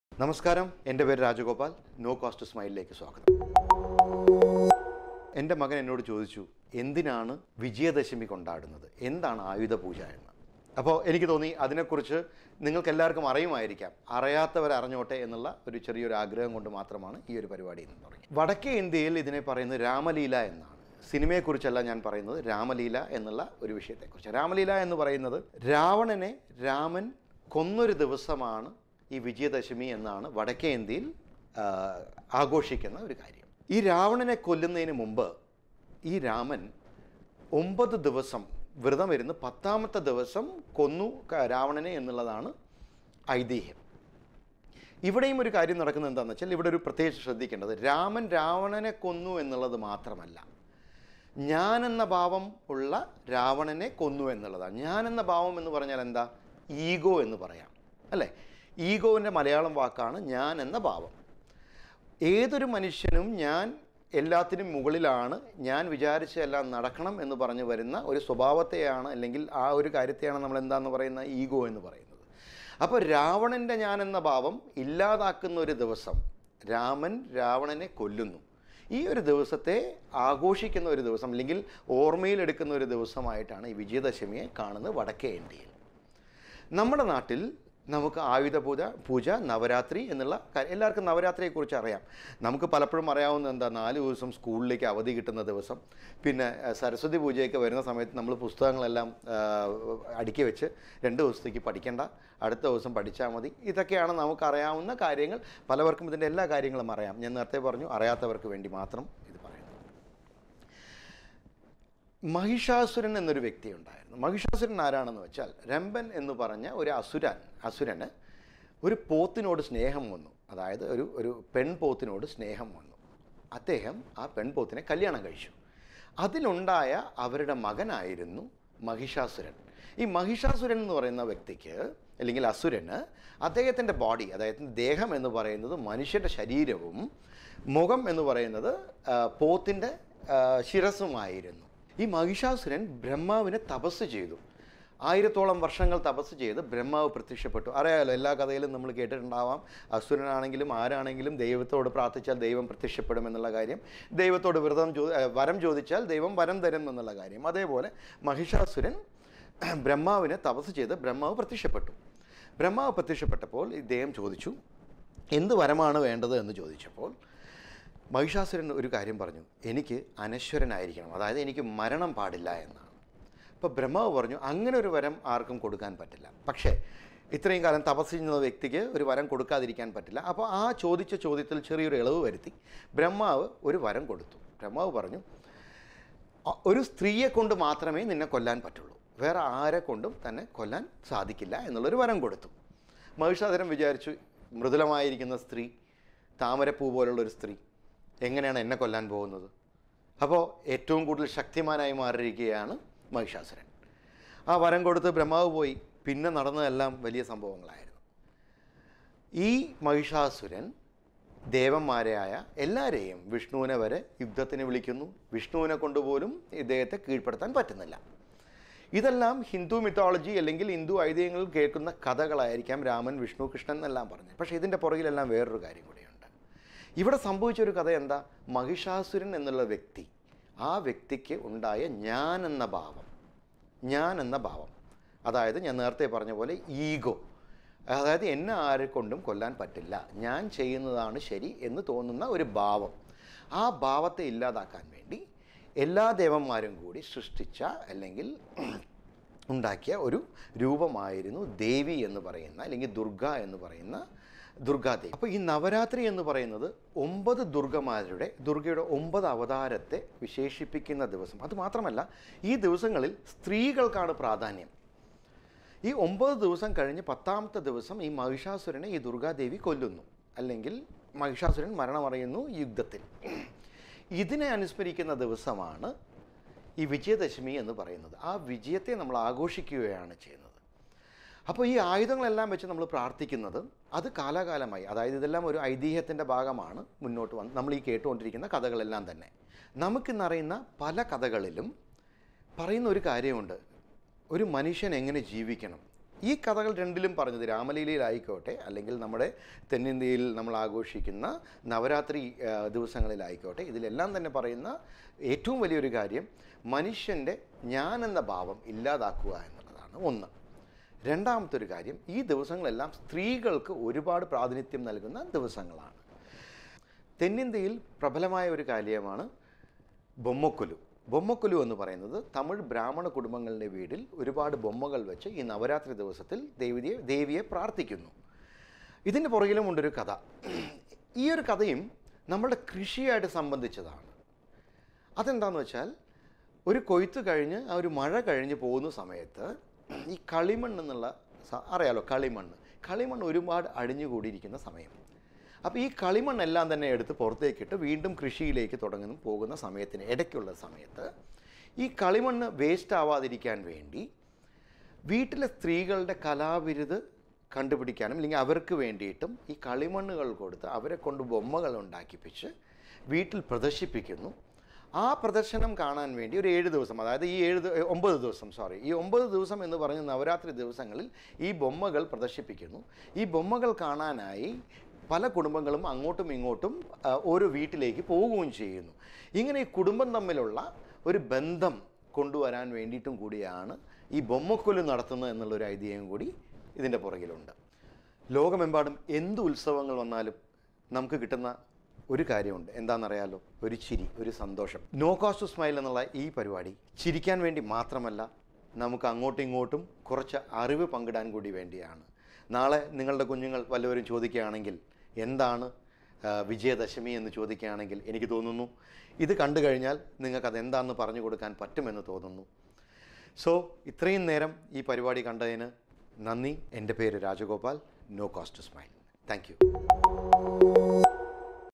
RAMA LEELA | Vidhayarambam | Ayudha pooja | രാമലീല വിദ്യാരംഭം | navaratri
Ayudha Pujai is an integral part of the Navratri festival (festival of triumph), a Hindu festival which is traditionally celebrated in India. It is also called "Astra Puja", the synonym for Ayudha Puja. In simple terms, it means “Worship of Instruments”.

no cost to smile, lets talk positive